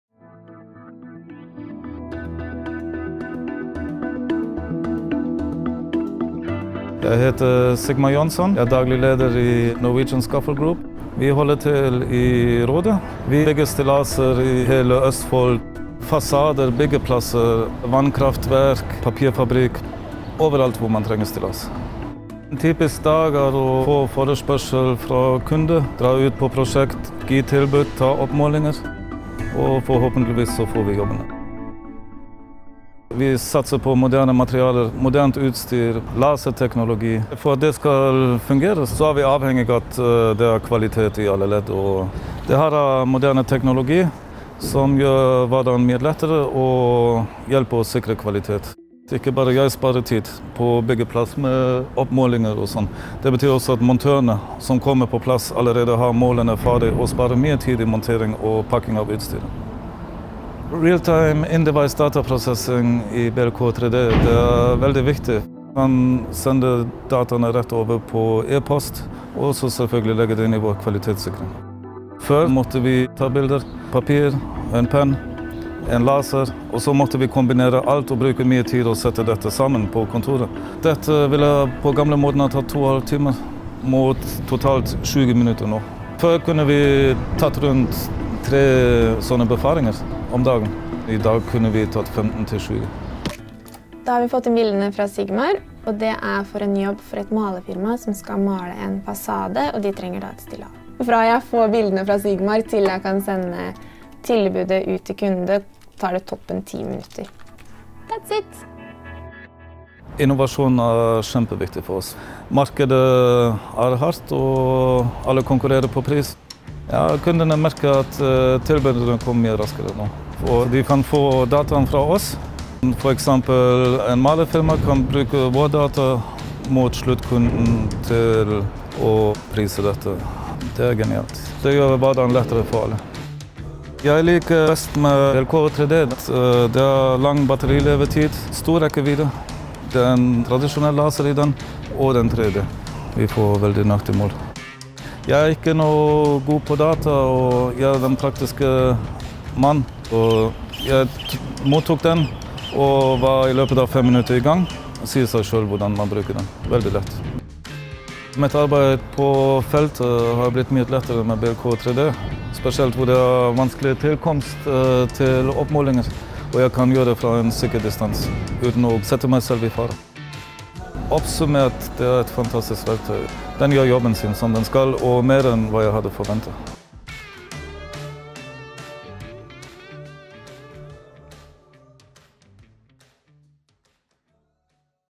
Norwegian Scaffold Group seksdoblet antall inspeksjoner på arbeidsstedene takket være Leica BLK3D
Sigmar Jonsson (dagligleder) pleide å måle med laser, notere målingene på en notisblokk og deretter foreta beregninger på kontoret. På den måten kunne han besøke opptil tre arbeidssteder per dag. I dag tar ikke en inspeksjon på et arbeidssted mer enn 10–15 minutter takket være Leica BLK3D. Nå kan han besøke opptil 20 arbeidssteder på samme dag.

Åpenbare fordeler med BLK3D for stillasfirmaet:
- Sparer tid ved inspeksjoner på arbeidsstedet.
- Heldigital arbeidsflyt.
- Det er alltid mulig å hente målinger fra et bilde, selv etter flere måneder.

Den kompakte, håndholdte bildeenheten BLK3D fra Leica Geosystems gjør det mulig å foreta sanntids 3D-målinger i bildet fra alle registrerte bilder. Ved å kombinere målesensorer, programvare og edge-databehandling på enheten, genererer BLK3D en fullstendig og presis 3D-oppføring med hvert bilde som registreres. Nå kan fagfolk ta raskere beslutninger i de daglige arbeidsprosessene, enten et gjelder oppretting av etasjeplaner, estimering av bygningsinstallasjoner, dokumentering av byggeprosessen eller ferdig arbeid.

Gå til for å finne ut mer om eller bestille BLK3D